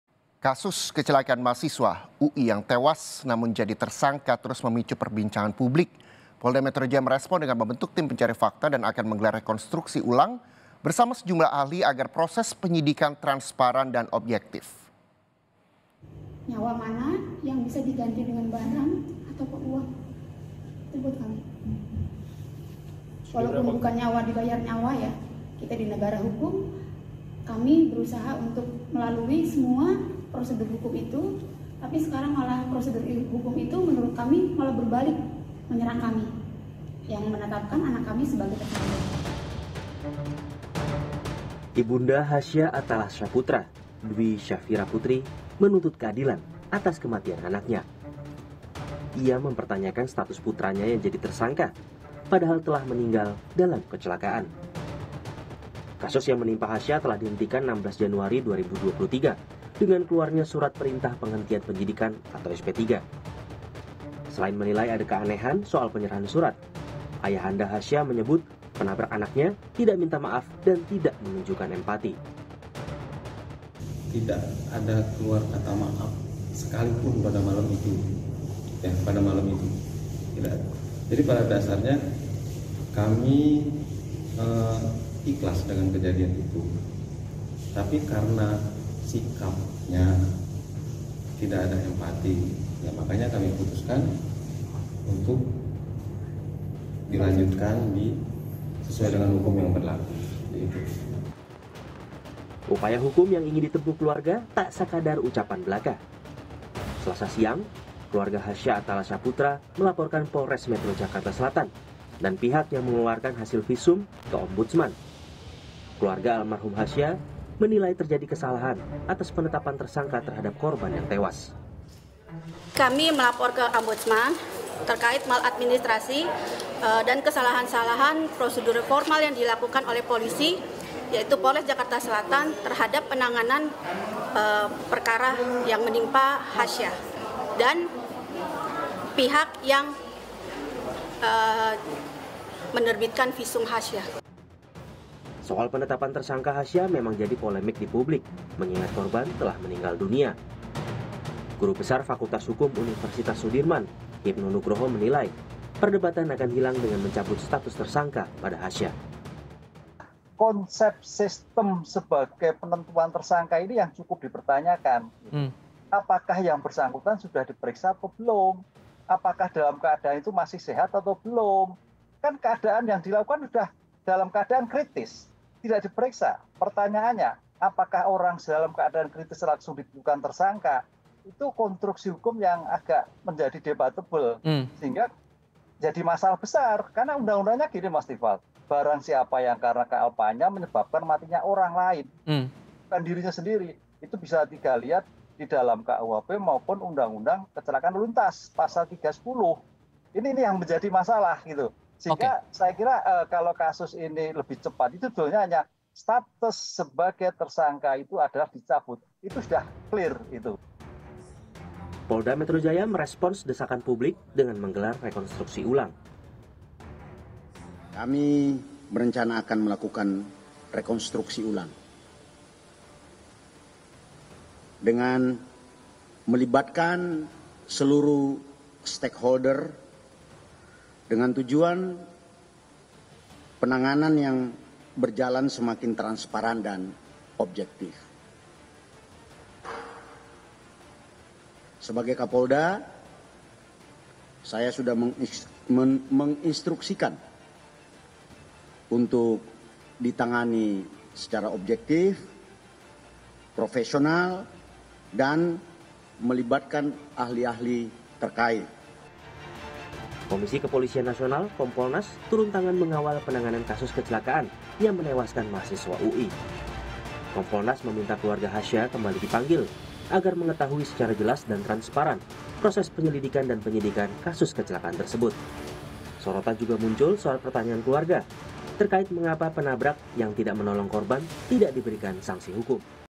Penuh Kejanggalan, Tim Pencari Fakta Telusuri Kasus Kecelakaan Mahasiswa UI Hasya Attalah Syahputra!
JAKARTA, KOMPAS.TV - Kasus kecelakaan Mahasiswa UI yang tewas, namun jadi tersangka, terus memicu perbincangan publik.

Polda Metro Jaya merespons dengan membentuk tim pencari fakta, dan akan menggelar rekonstruksi ulang bersama sejumlah ahli agar proses penyidikan transparan dan obyektif.

Di sisi lain, Ibunda Hasya Attalah Syahputra, Dwi Syafiera Putri menuntut keadilan atas kematian anaknya.

Ia mempertanyakan status putranya yang jadi tersangka, padahal telah meninggal dalam kecelakaan.

Kasus yang menimpa Hasya telah dihentikan 16 Januari 2023 dengan keluarnya Surat Perintah Penghentian Penyidikan (SPPP).

Selain menilai ada keanehan soal penyerahan surat, Ayahanda Hasya menyebut penabrak anaknya tidak minta maaf dan tidak menunjukkan empati.

Upaya hukum yang ingin ditempuh keluarga, tak sekadar ucapan belaka.

Selasa (31/1) siang, keluarga Hasya Atalla Syahputra melaporkan Polres Metro Jakarta Selatan dan pihak yang mengeluarkan hasil visum ke Ombudsman.

Keluarga almarhum Hasya menilai terjadi kesalahaan atas penetapan tersangka  terhadap korban yang tewas.

Soal penetapan tersangka hasya memang jadi polemik di publik mengingat korban telah meninggal dunia.

Guru Besar Fakultas Hukum Universitas Sudirman, Hibnu Nugroho menilai, perdebatan akan hilang dengan mencabut status tersangka pada Hasya.